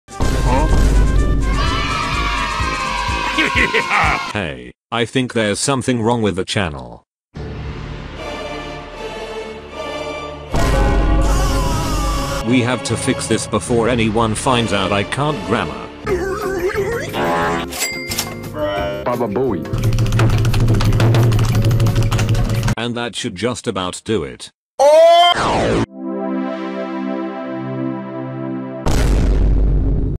lochlann fixes his username [3rd aniversary]
lochlann corrects his youtube username so he dosnt get arrested by the grammar police